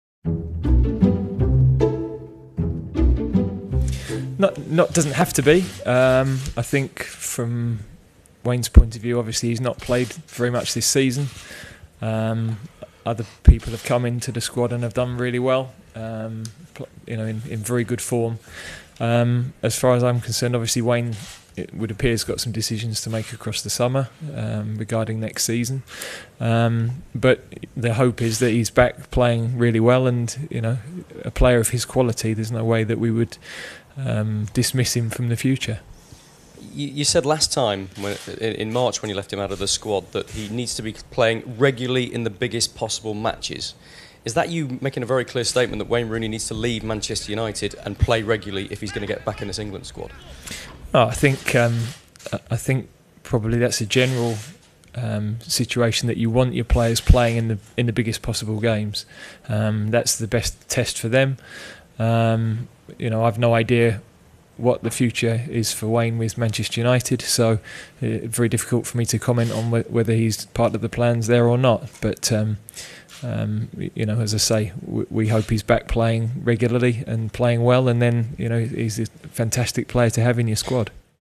Gareth Southgate comments on Wayne Rooney's performance in May - Daily Mail
EXCLUSIVE: Wayne Rooney is giving serious consideration to seeing out the rest of his contract at Manchester United, which Sportsmail can reveal has just one year to run with the option of a further year. While Rooney will not make a final decision on his future until he returns from a family holiday &#8212; the transfer window here and in China opens at the start of July &#8212; it is understood the choice to stay at Old Trafford is becoming increasingly attractive. A return to Everton is looking unlikely, not least because any interest from the Goodison Park club does not extend to going anywhere close to matching Rooney&#8217;s current £250,000-a-week wage.